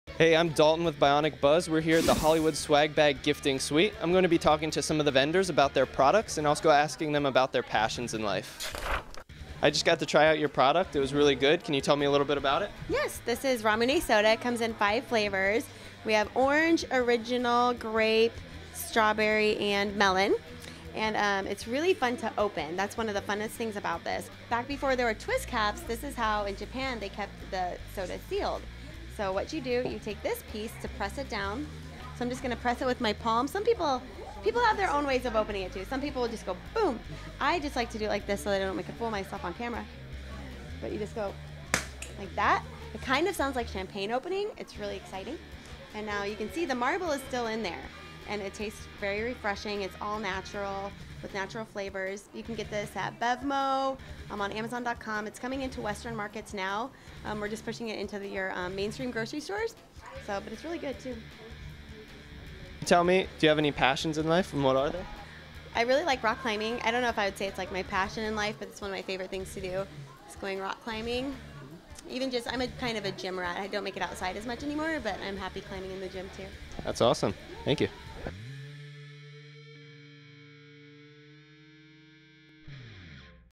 Ramune Soda Interview & Review by Bionic Buzz
Bionic Buzz attended the Hollywood Swag Bag gifting suite at the Four Seasons Hotel in Beverly Hills, CA. The event was to (sadly) celebrate the end of the summer and the 2016 Teen Choice Award winners. We got to check out Ramune, a sweet & refreshing Japanese soda that comes in a wide variety of flavors.